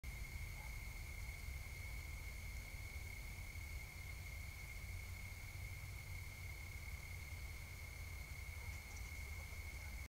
Gray Fox Ambush
A Gray Fox Dog surprises his mate, a pretty Gray Fox Vixen, who isn’t quite ready for breeding.

Trail Camera: BushNell Trophy Cam HD Aggressor